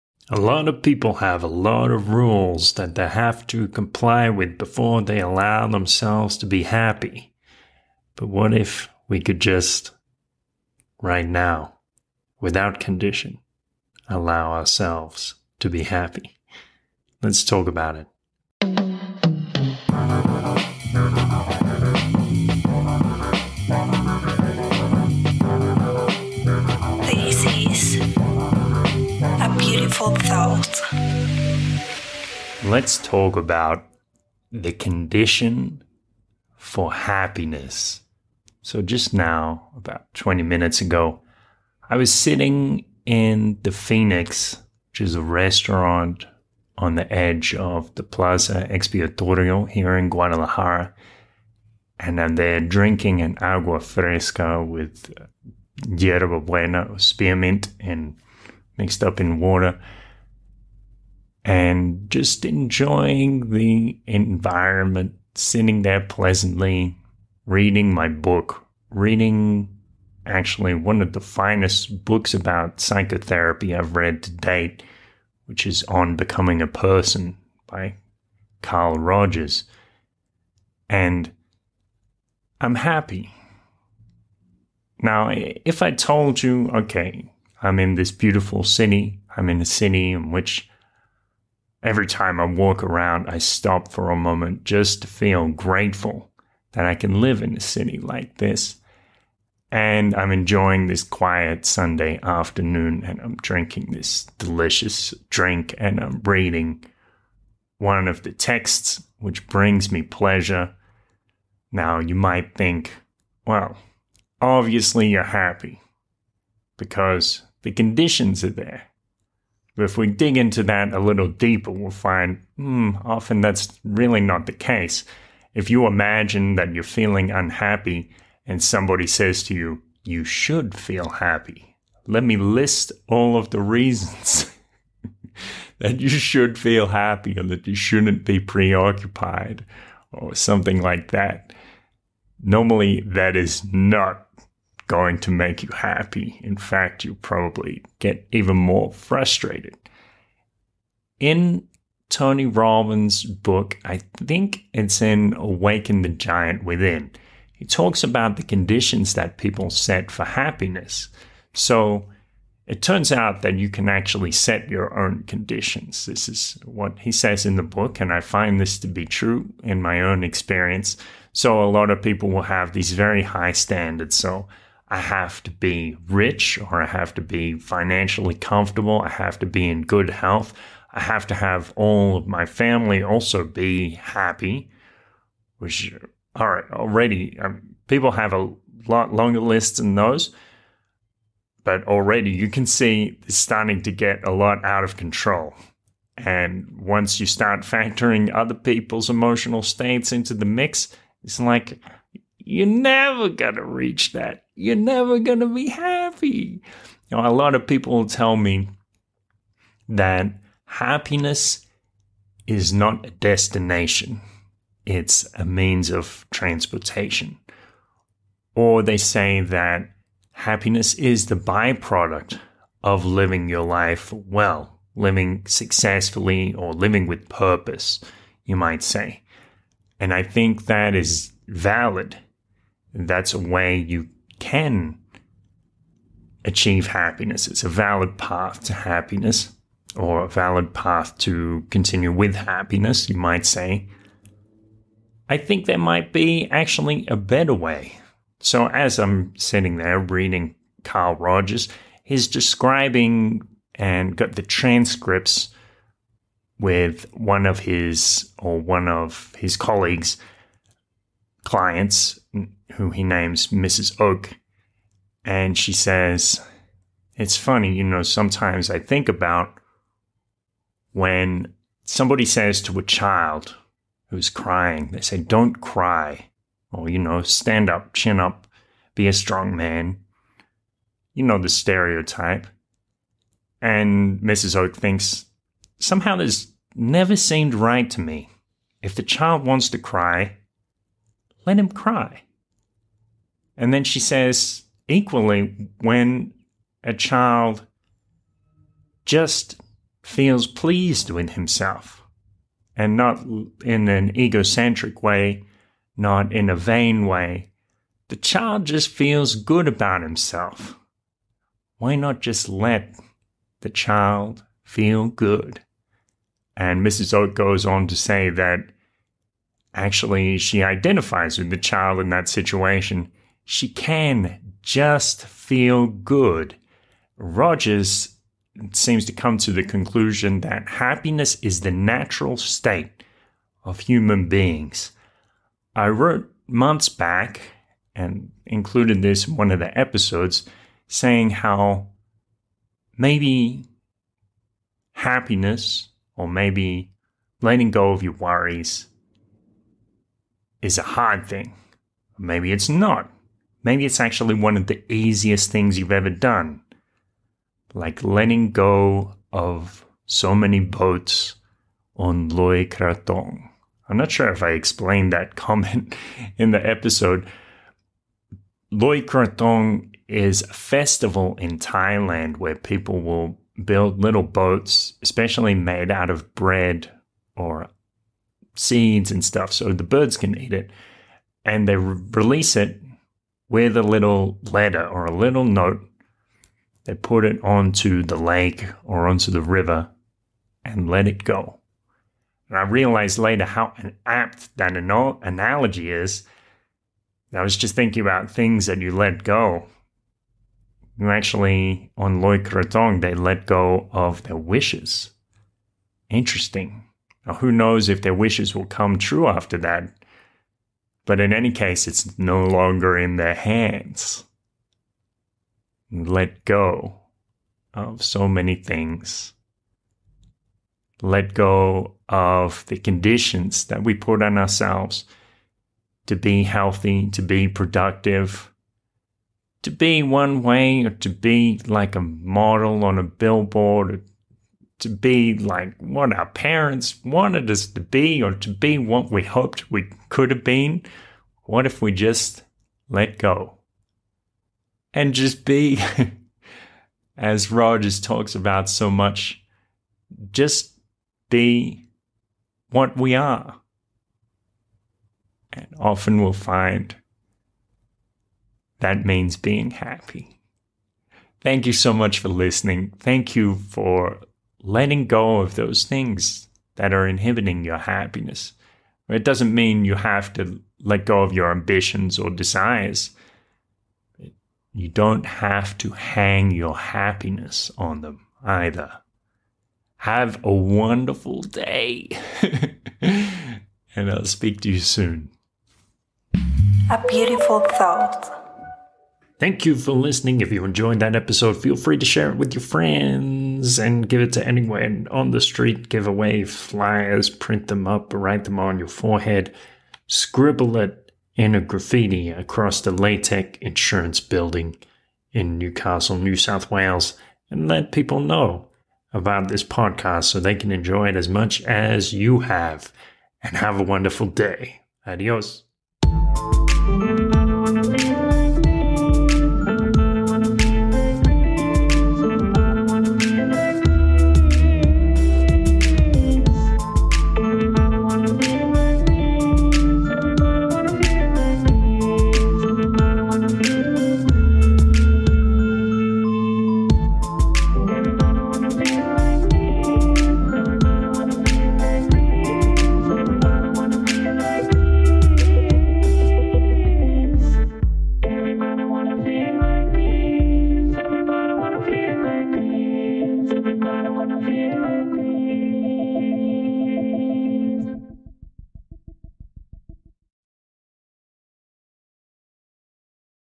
Conditions for Happiness - 98 - A Beautiful Thought
In Tony Robbins’ books, he describes how many people have very high standards for happiness. Everything has to be in line before they will allow themselves to be happy – their work, their love life, their family life.

Of course, the higher the standards we set for happiness, the less likely we are to achieve it. As Robbins points out, there is nothing stopping us from deciding to have low standards for happiness – for deciding that being with friends, dancing, eating, breathing or anything else are the only conditions we require to be happy.

In “On Becoming A Person”, humanist psychologist Carl Rogers describes the case of a client named “Mrs. Oak”. Mrs. Oak describes her revelation, noticing that it never seems quite right when parents tell a child not to be sad, not to cry. If the child wants to cry, says Oak, just let him cry.

Likewise, if a child feels like being happy, being pleased with himself – not in an egotistical or vain way – we can let him be happy. Equally, we can let ourselves be happy.